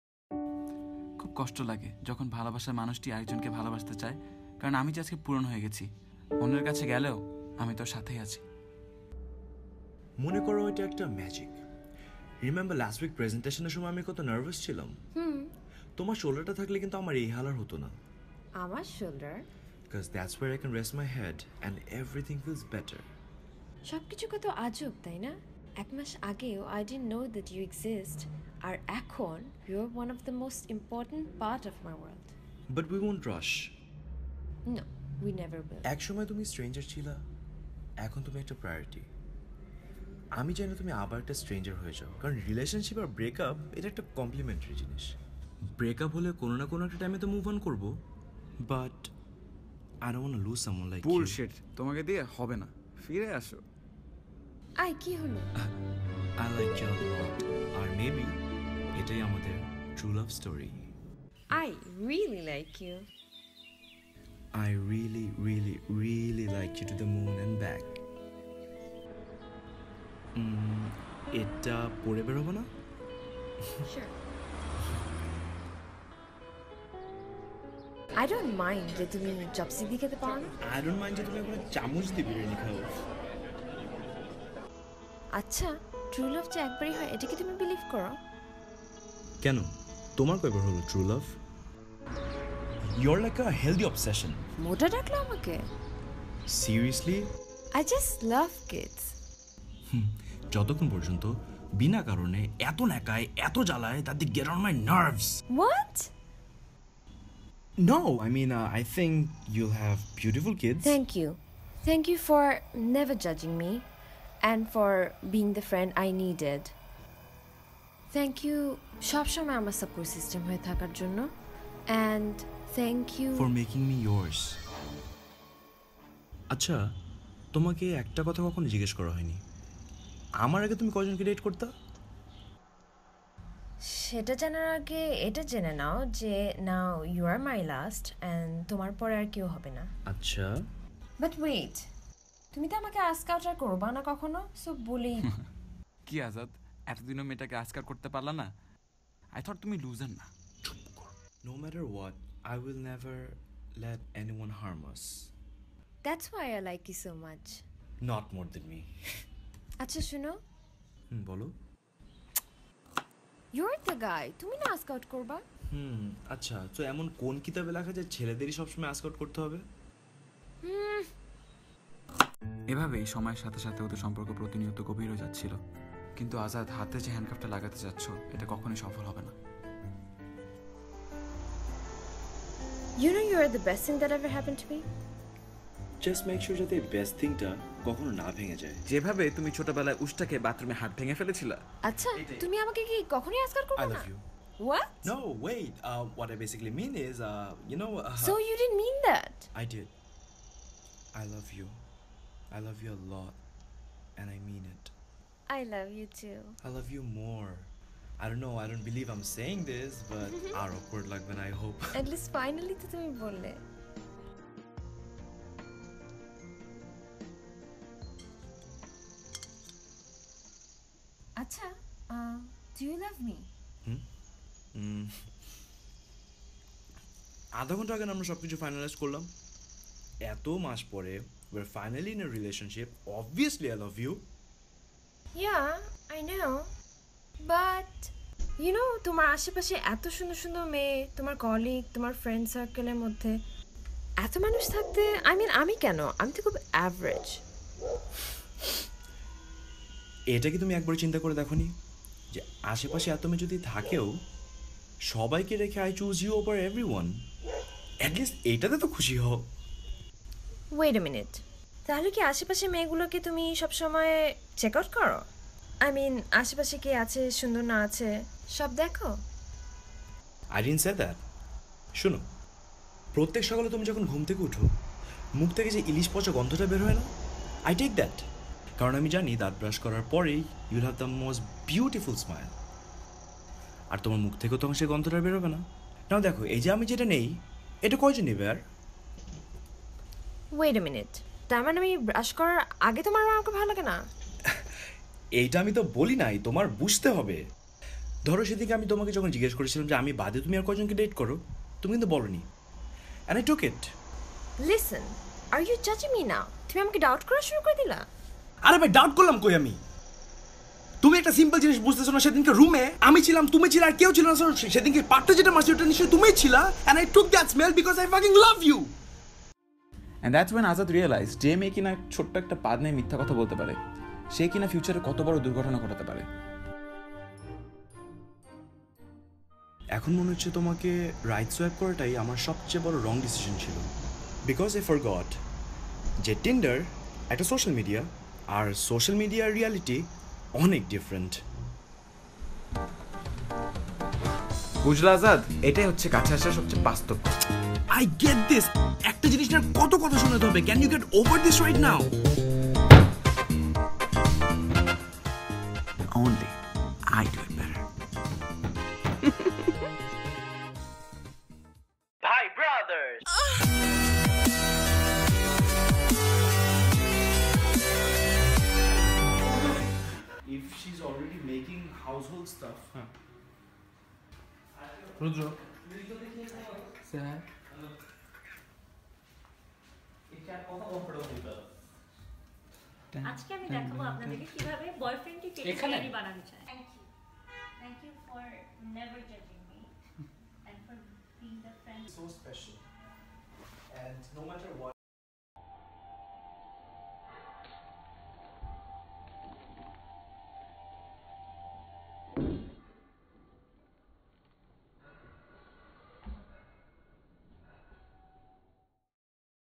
A True Love Story - BhaiBrothers LTD.
14 feb is our third year of youtubing and we wanted to come back with a bhaibrothers classic. subscribing is an adorable way of showing love, for free, on valentine's day! love you good people

Instagram / Snapchat / Twitter : ChotoAzad

thanks to safat for the cinematography
gratitude to irtifa for being an average friend
thanks to rudro for letting me edit the video